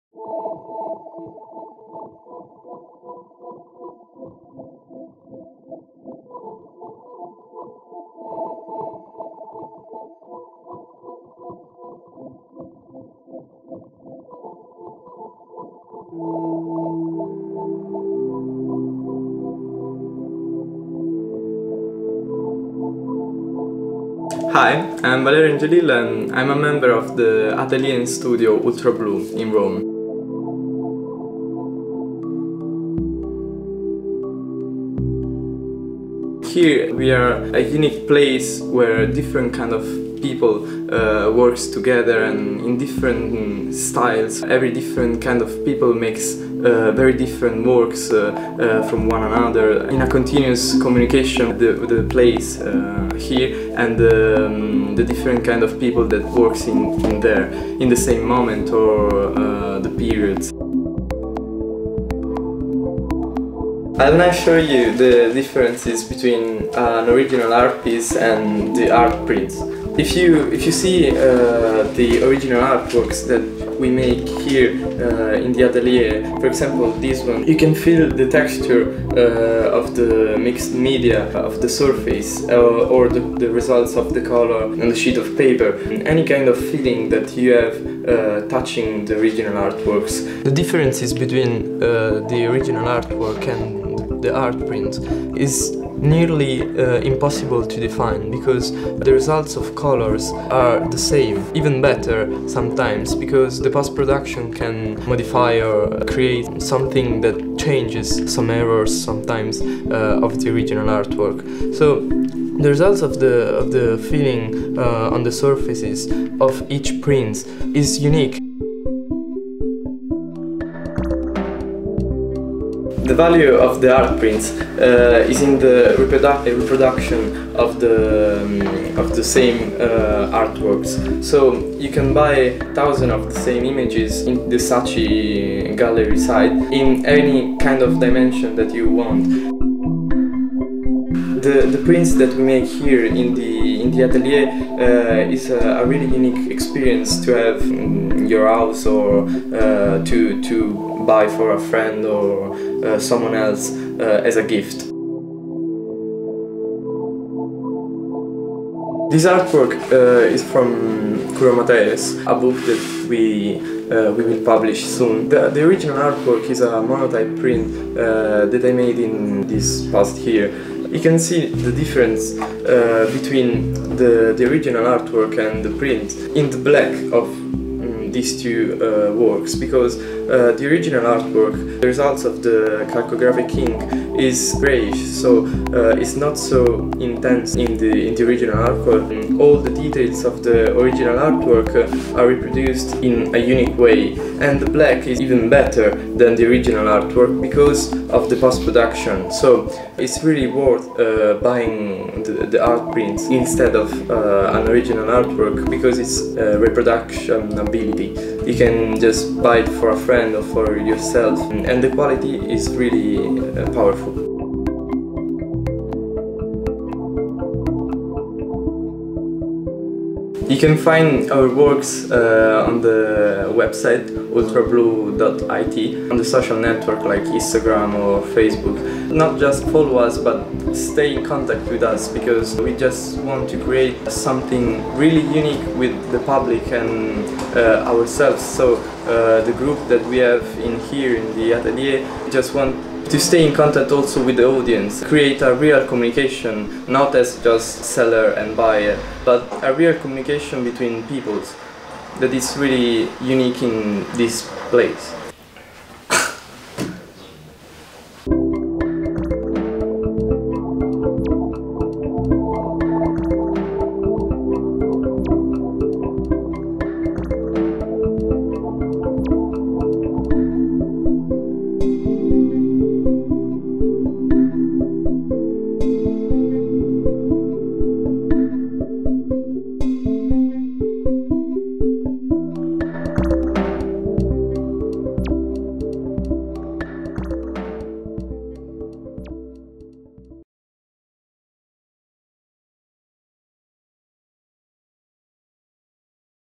Valerio Angiolillo Fine art prints
Valerio Angiolillo Fine art prints. These are faithful and very accurate reproductions of the artworks created at Ultrablu atelier and thanks to the very high quality of the prints, it will be like having the original in your hands.
Stampe d'arte di Valerio Angiolillo. Si tratta di riproduzioni fedelissime delle opere fatte all’interno di Ultrablu, con l’altissima qualità delle nostre stampe sarà come avere un originale tra le mani.